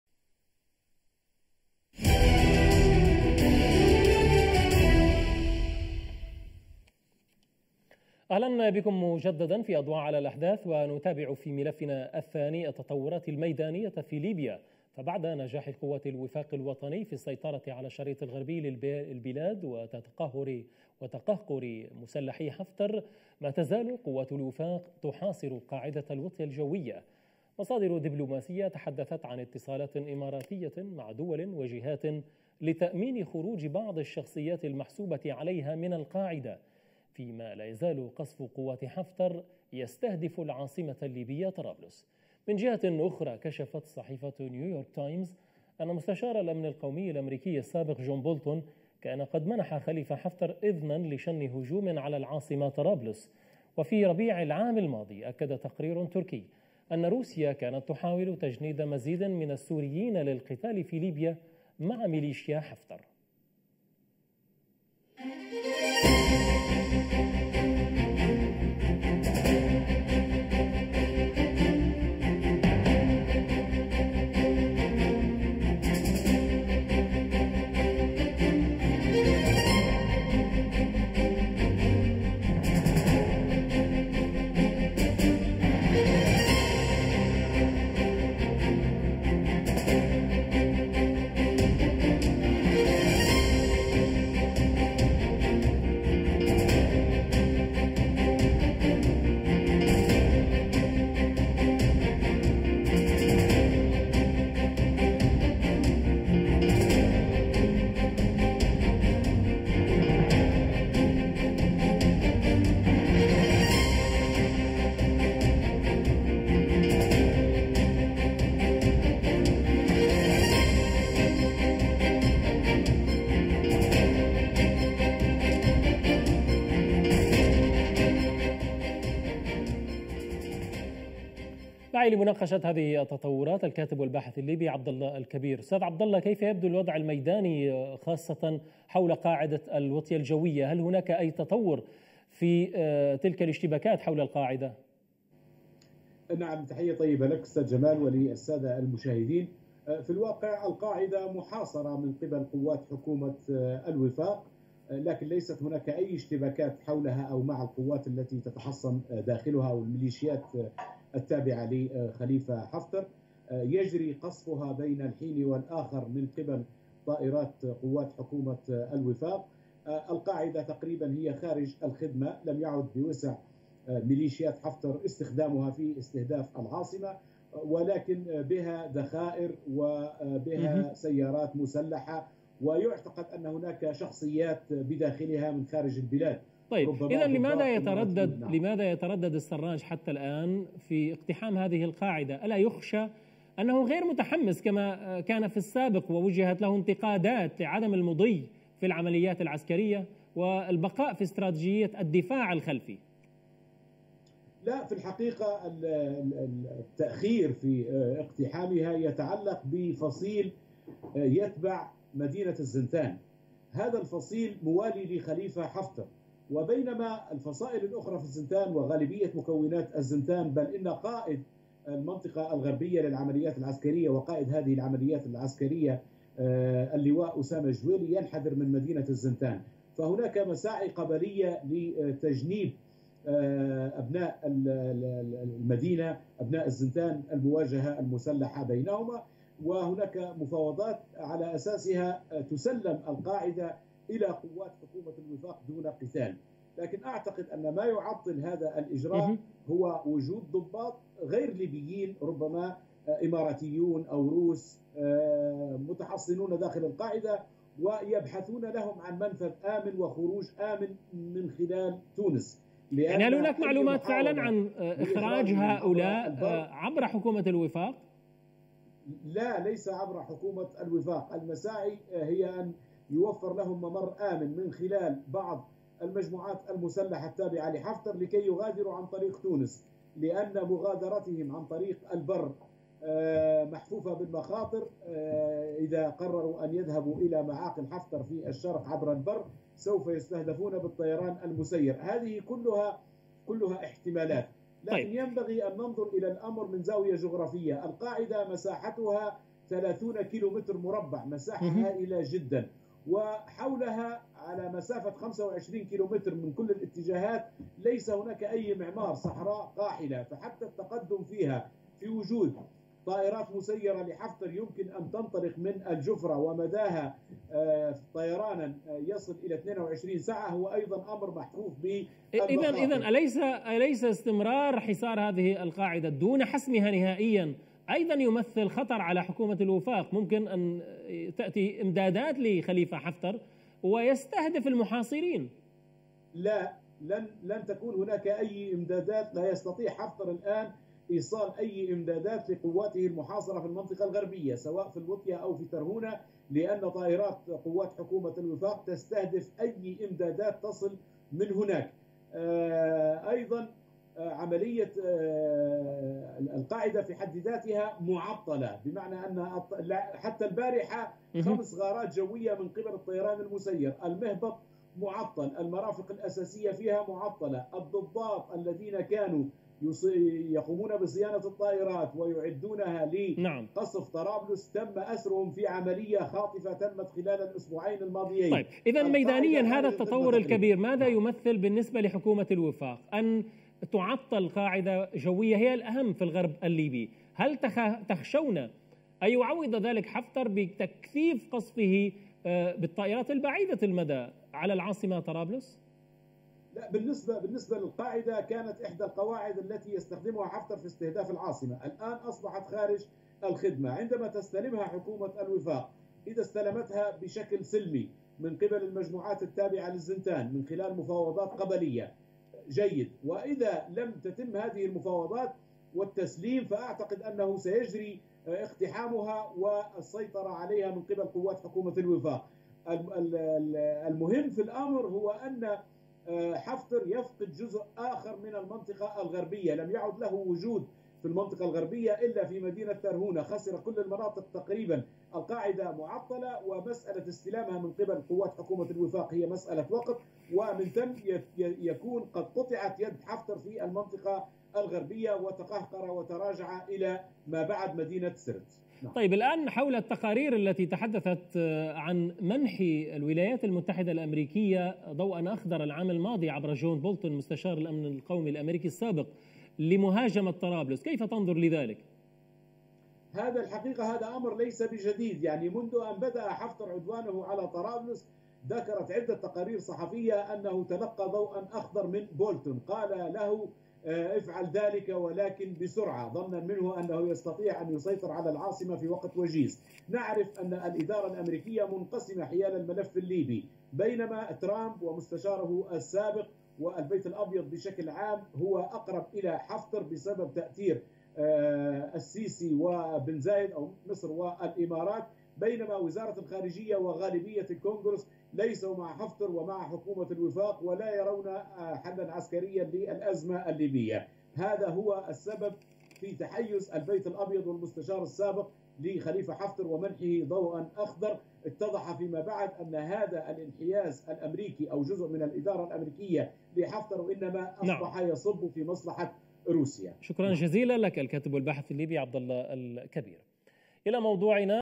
ليبيا: قوات الوفاق تحاصر قاعدة "الوطية "ومساع إماراتية لإنقاذ حفتر 🇱🇾
أضواء على الأحداث
تقديم : جمال أحمد